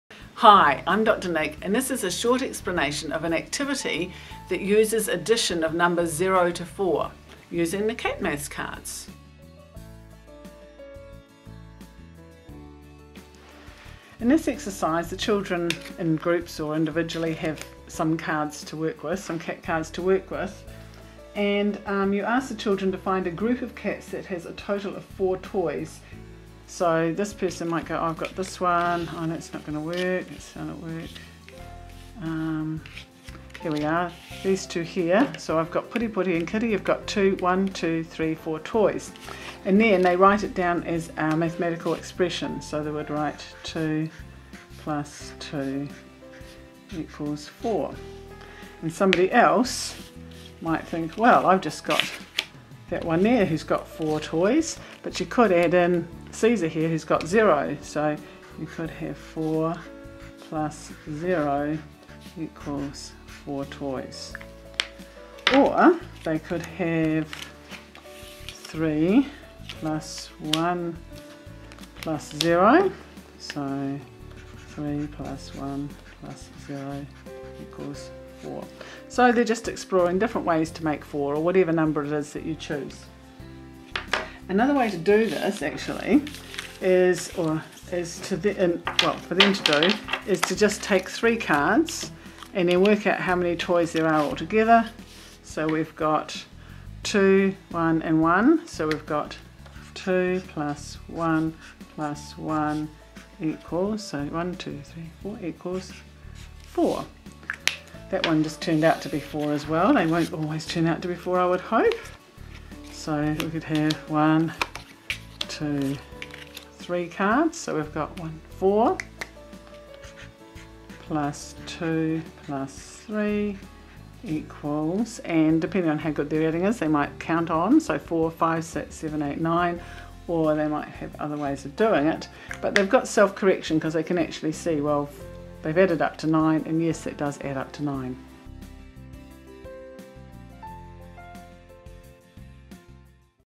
Cat Maths: Addition of numbers 0 to 4
This video shows two different activities that teaches and practices addition of small numbers using Cat Maths cards. The first activity also teaches that there are multiple expressions that are equal.